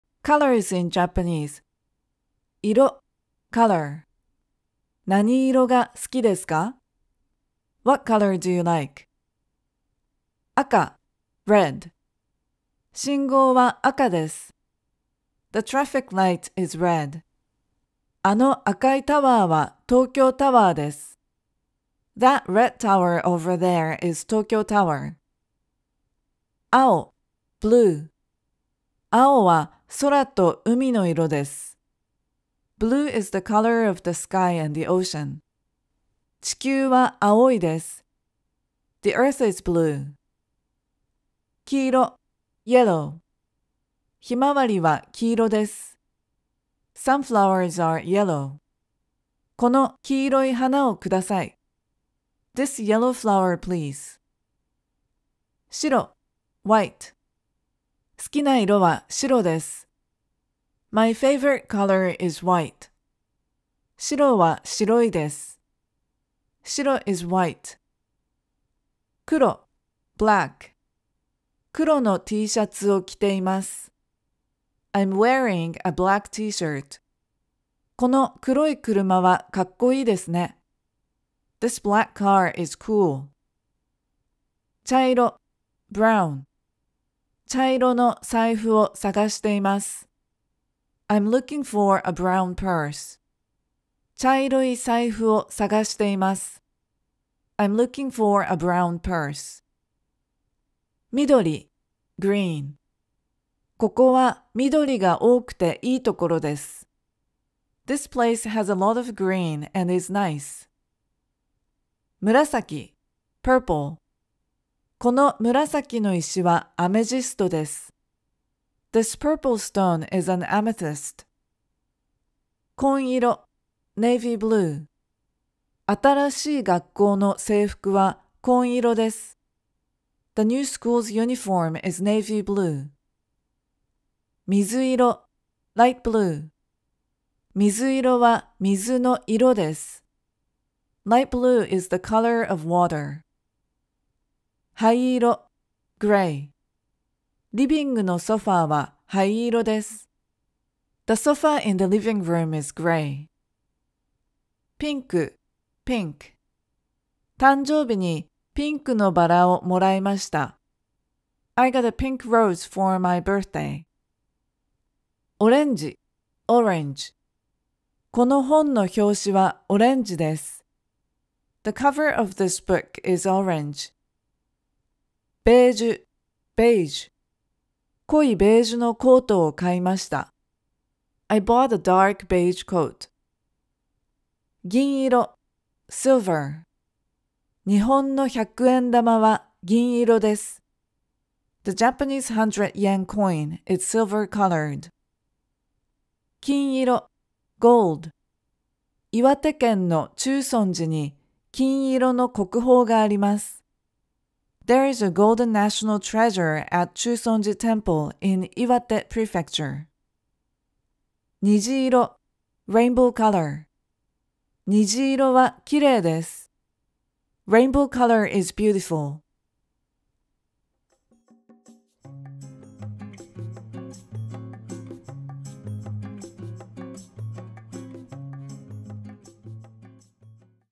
[GENKI Lesson 9] Colors in Japanese
In this video, we learn colors（色：いろ） in Japanese! 何色（なにいろ）が好（す）きですか？What color do you like?

0:00 color
0:10 red
0:26 blue
0:41 yellow
0:54 white
1:08 black
1:24 brown
1:40 green
1:52 purple
2:02 navy blue
2:13 light blue
2:24 gray
2:34 pink
2:44 orange
2:54 beige
3:04 silver
3:15 gold
3:30 rainbow color